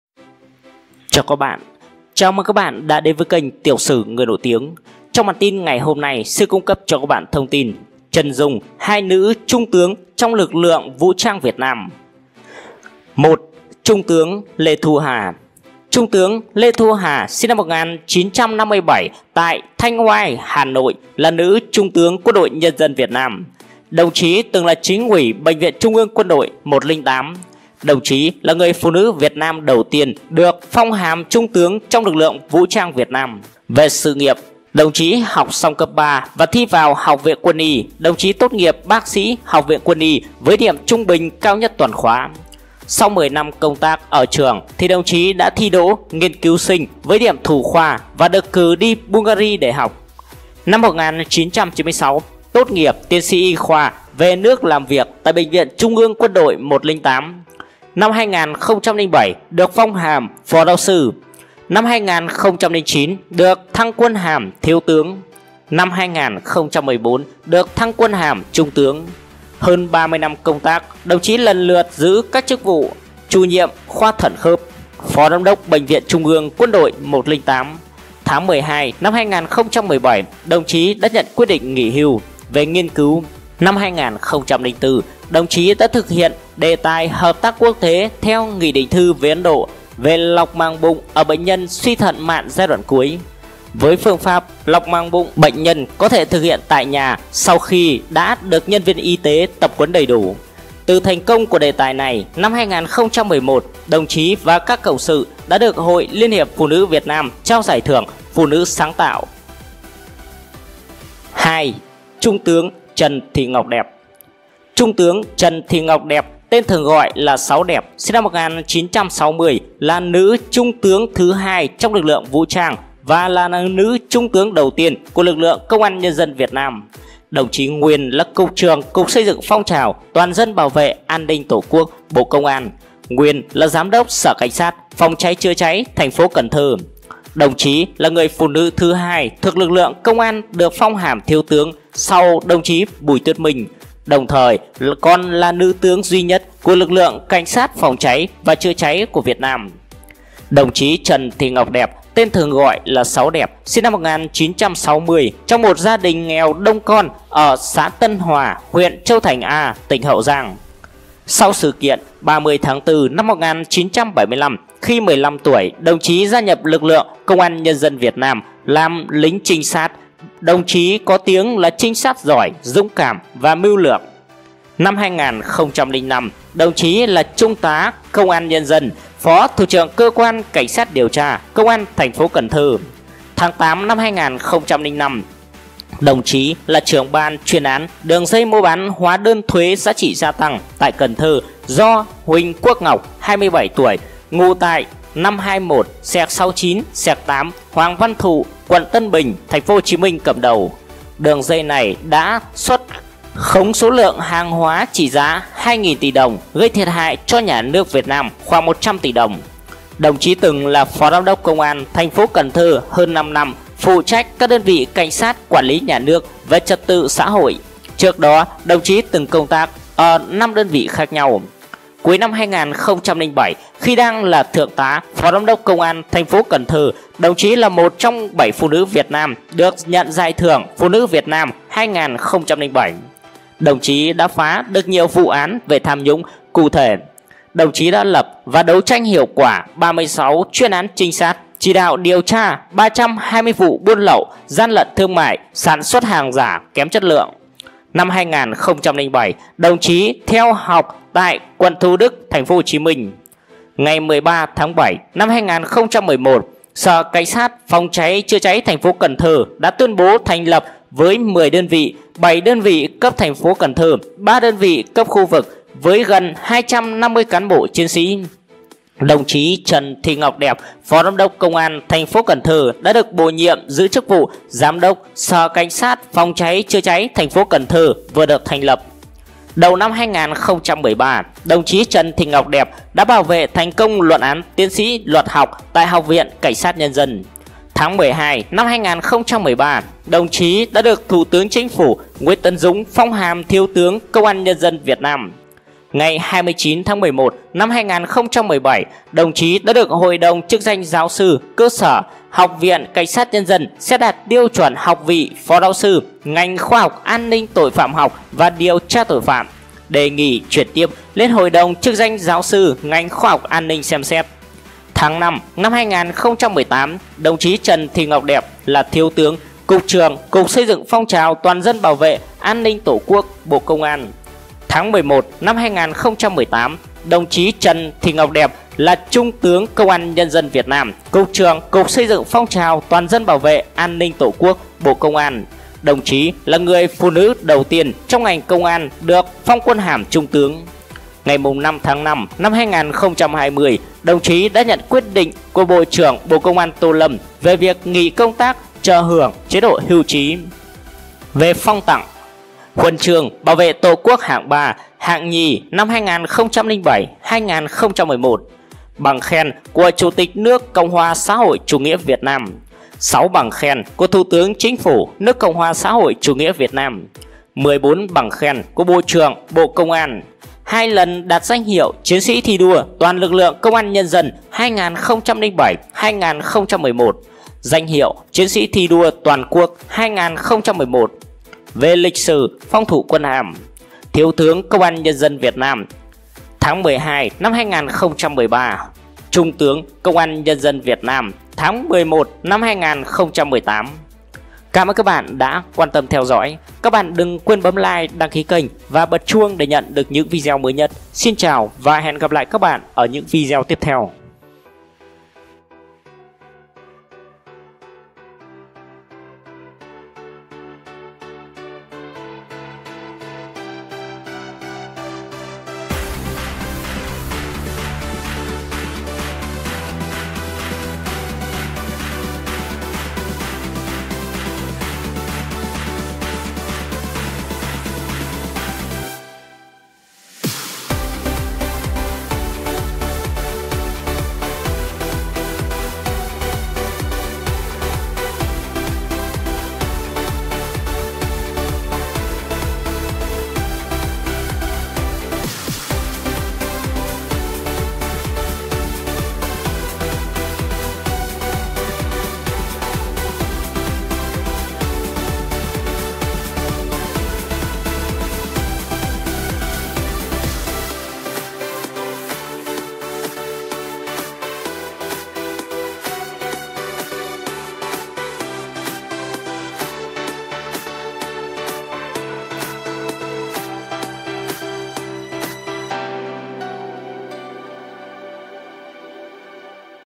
Chân Dung 2 Nữ Trung Tướng Lực Lượng Vũ Trang Việt Nam Qua Các Thời Kỳ
Lê Thu Hà là Trung tướng Quân đội nhân dân Việt Nam. Bà từng là Chính ủy Bệnh viện Trung ương Quân đội 108. Bà là người phụ nữ Việt Nam đầu tiên được phong quân hàm Trung tướng trong Lực lượng vũ trang Việt Nam
Trần Thị Ngọc Đẹp là nữ Trung tướng đầu tiên của lực lượng Công an nhân dân Việt Nam. Bà nguyên là Cục trưởng Cục Xây dựng Phong trào Toàn dân Bảo vệ An ninh Tổ quốc, Bộ Công an; nguyên là Giám đốc Sở Cảnh sát Phòng cháy chữa cháy Thành phố Cần Thơ